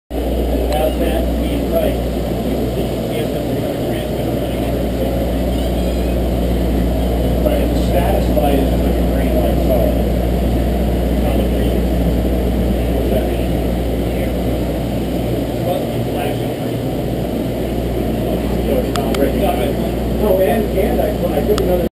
Raymarine Trailer
Demo trailer by Raymarine displaying their newest and greatest electronics at the West Marine Sarasota Flagship